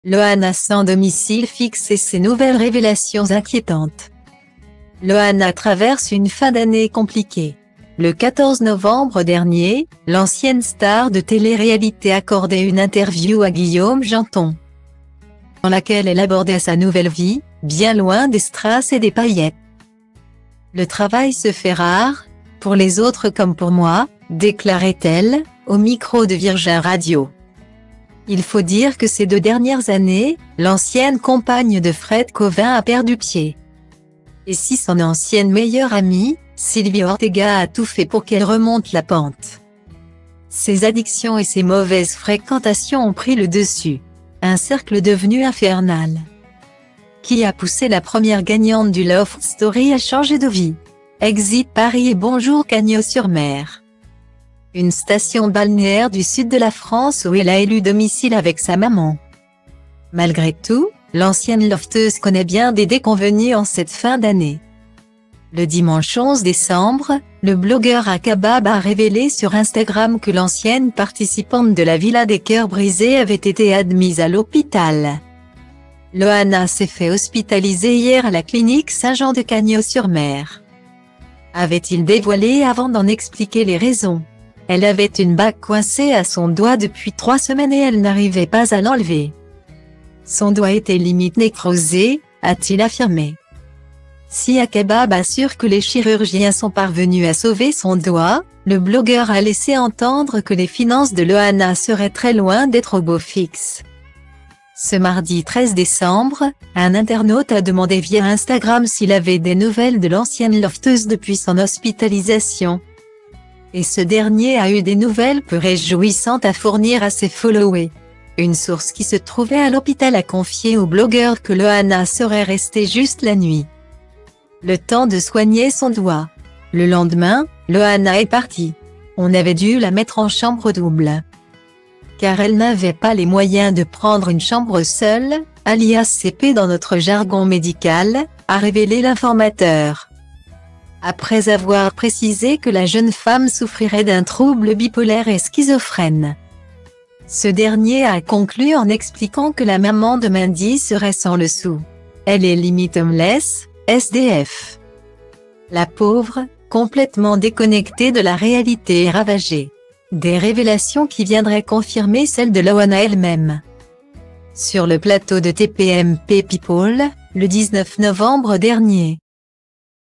Loana « sans domicile fixe » et Ces nouvelles révélations inquiétantes
Loana traverse une fin d'année compliquée. Le 14 novembre dernier, l'ancienne star de télé-réalité accordait une interview à Guillaume Genton,dans laquelle elle abordait sa nouvelle vie, bien loin des strass et des paillettes."Le travail se fait rare, pour les autres comme pour moi", déclarait-elle, au micro de Virgin Radio.